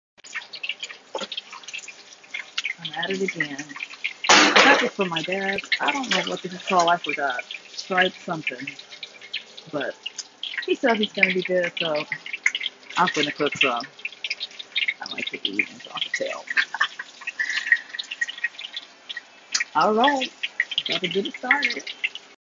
Frying fresh saltwater stripe bass fish...Dad scaled & cleaned it for me.
Ok, so I couldn’t remember what type of fish I was about to cook, but I finally got it. It was so good.